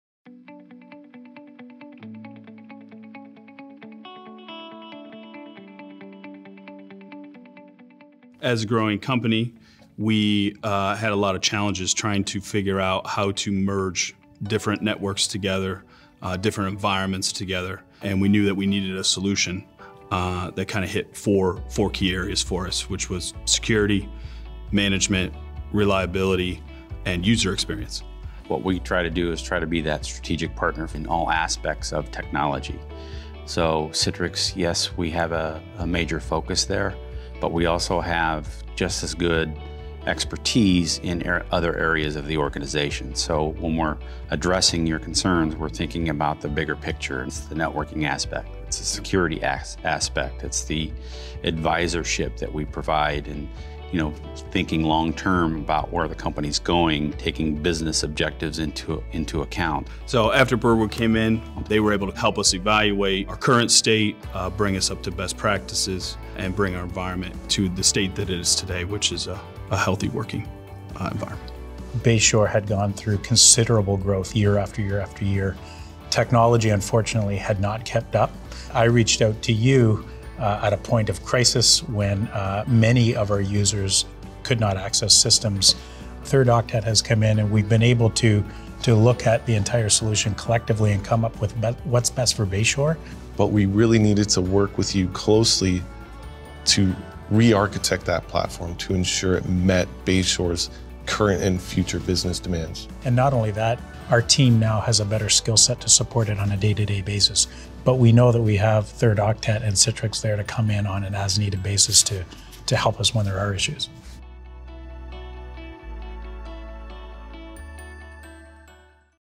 Why Customers Choose Citrix Partners
Already a partner? Explore more resources
Not yet a partner?
Citrix Partners deliver innovative technology solutions worldwide so their customers can work better and more securely anytime, anywhere, on any device. Hear how Citrix Partners Burwood Group and Third Octet are helping their customers overcome their IT challenges.